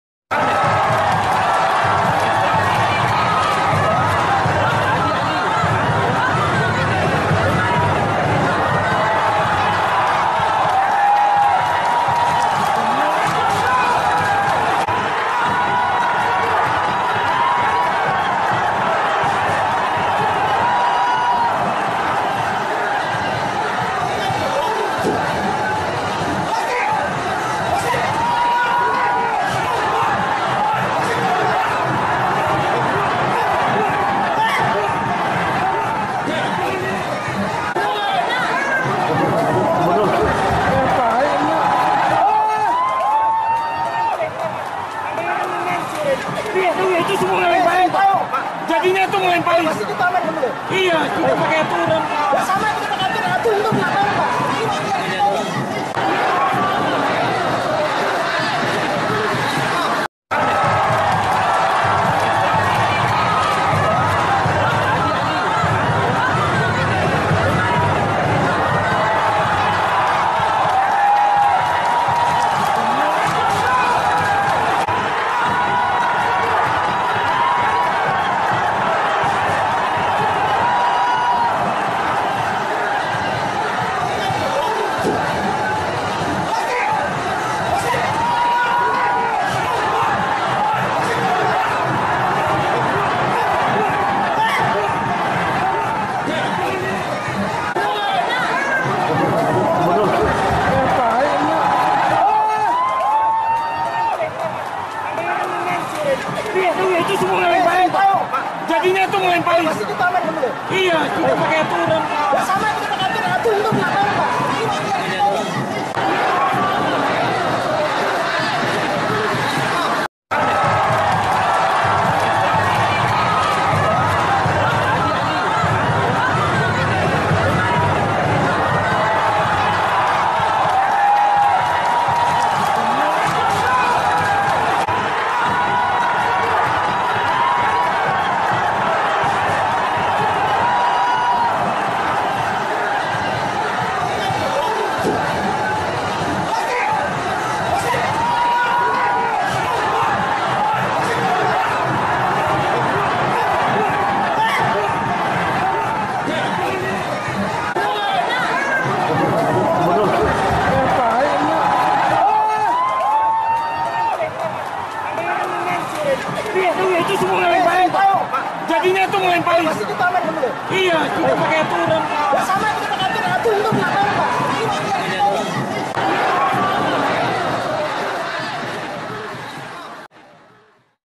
Begini Kalau Preman Jadi Gubernur - Gubernur Kalteng Tantang Duel Kapolres Palangkaraya
Gubernur Kalteng, Sugianto Sabran tantang duel Kapolres Palangkaraya, AKBP. Timbul Siregar